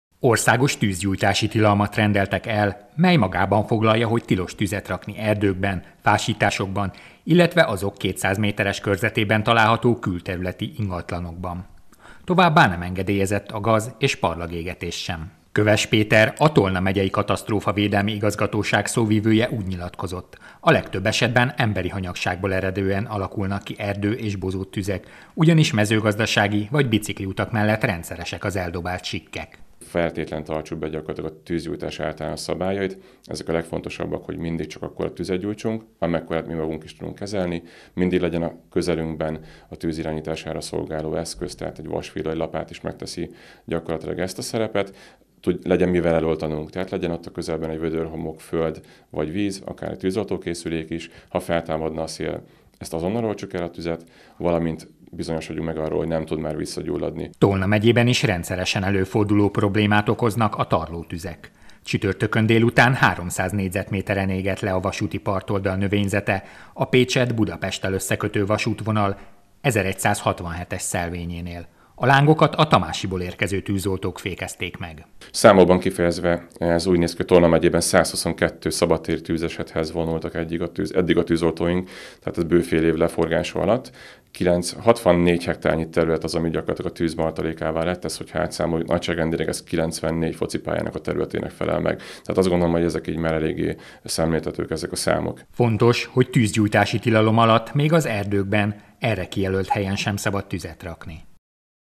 Tűzgyújtási tilalom - az egész ország területére vonatkozó korlátozást rendeltek el
Július másodikától tűzgyújtási tilalmat rendeltek el az egész ország területére. A szigorításra a kánikula és a szárazság miatt volt szükség. A hétvégén két esethez is riasztották a tűzoltókat Tolna megyében, Bonyhád külterületén a lekaszált fűszéna gyulladt ki, Gyulajon pedig avar kapott lángra a melegben.